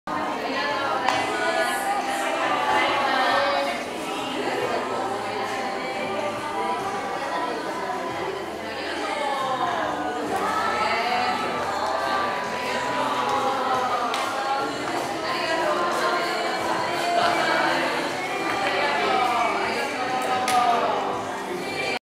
일본 크리스챤스쿨(초등)의 양로원방문/사랑의 인사
사랑의 악수/ 할렐루야 인터내셔널 크리스챤 스쿨의 양로원방문.

너무나 외로운 노인분들... 특히 마음의 표현을 잘 하지 않는 노인분들이었다.

악수...그것만으로도 사랑을 전하기에 충분했다.
예수님께서 우리의 몸을 통해 그들에게 다가가셨다.
크리스마스에 주께서 가장 바라시는 것은 무엇인가...

기쁘다 구주가 오셨다.